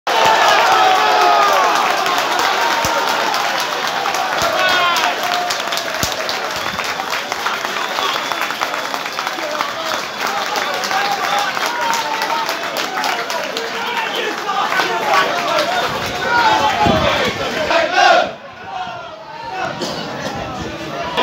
BOXING DAY GROUND HOP WITH THE DAGENHAM AND REDBRIDGE FANS
A boxing day ground hop to dagenham  on boxing day  it was a one all draw